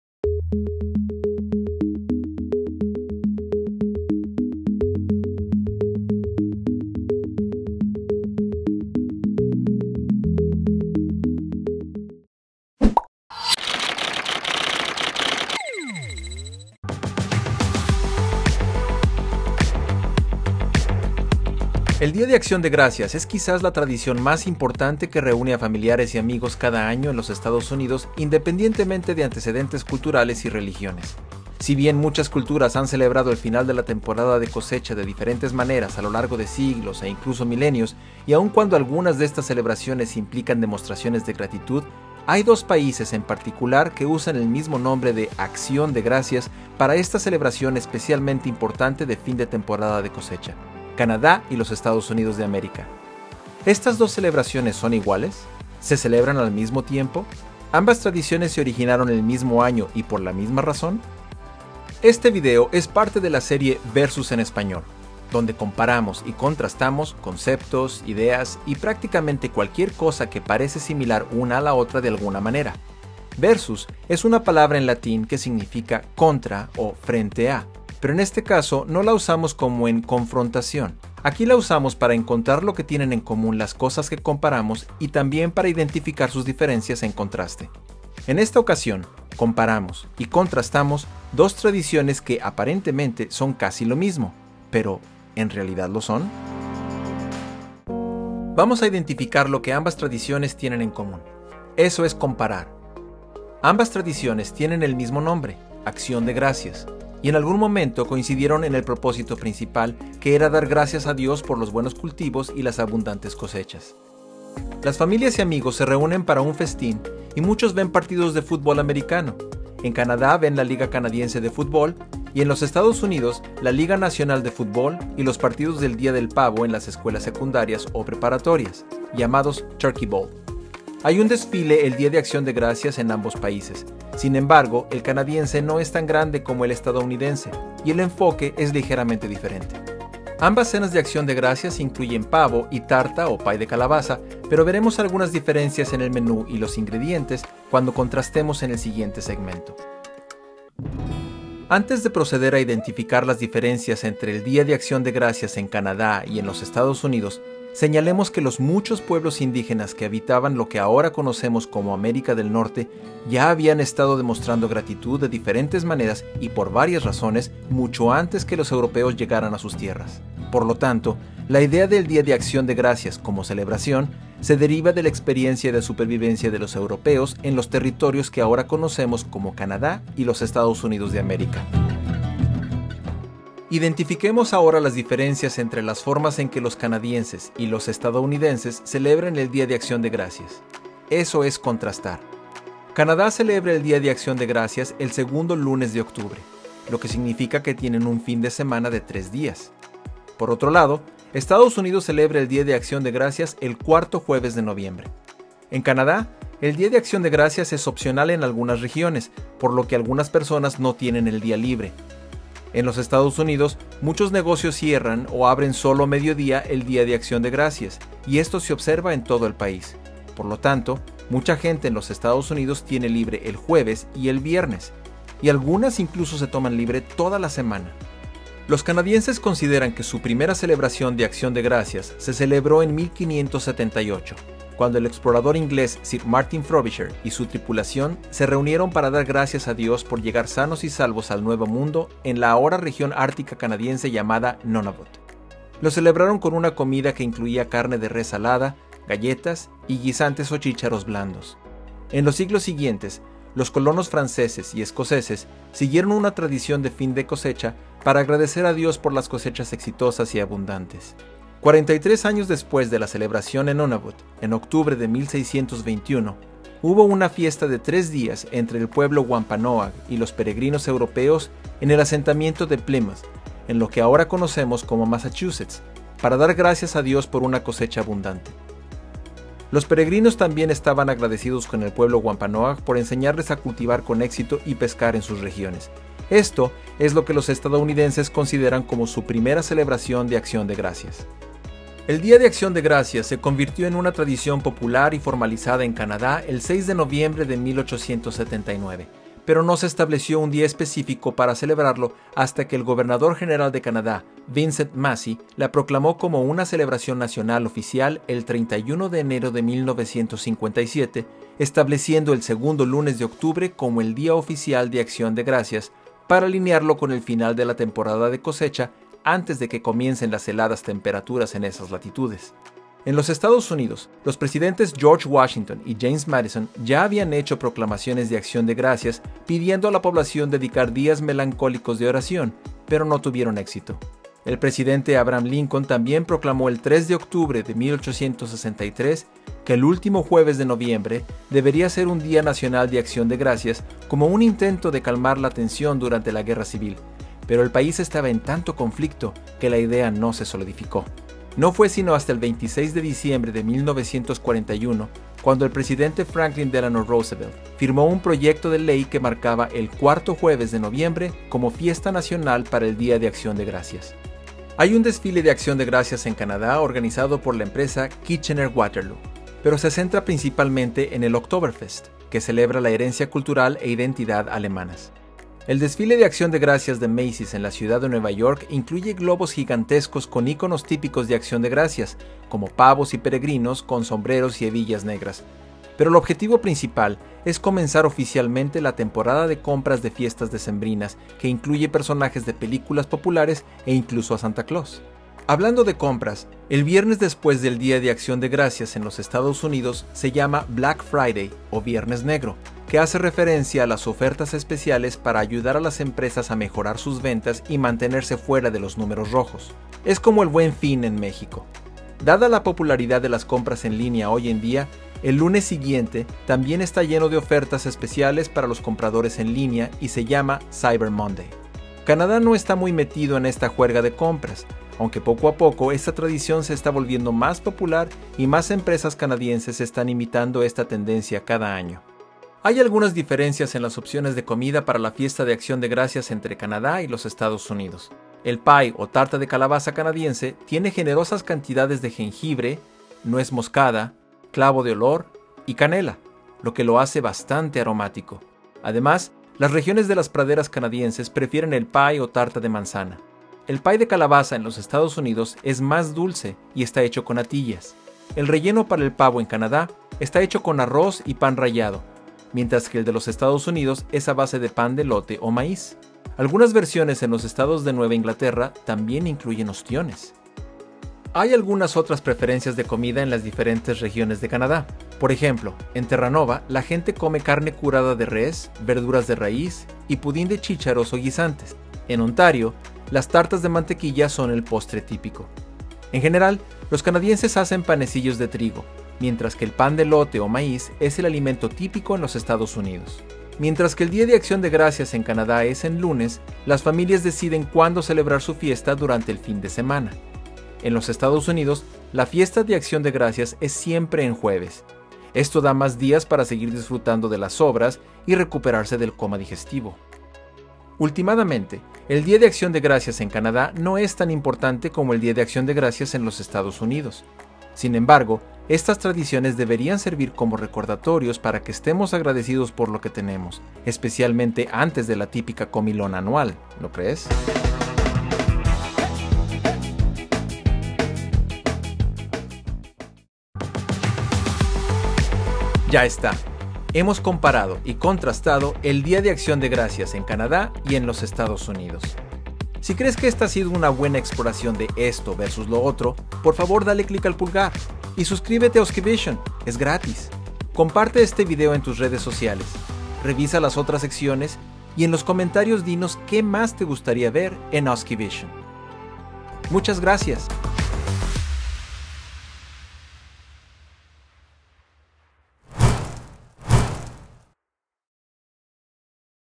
Acción de Gracias CAN vs E.E.U.U.
Una breve identificación de las similitudes y diferencias entre el Día de Acción de Gracias en Canadá y en los Estados Unidos